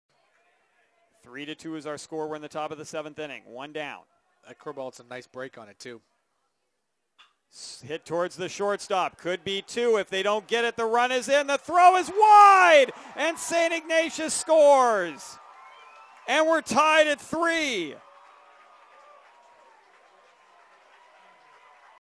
St. Inatius ties the game in the 7th
St. Ignatius ties the game on the Willow Glen error in the 7th Inning of the 2013 CIF Central Coast Section D-II Championship.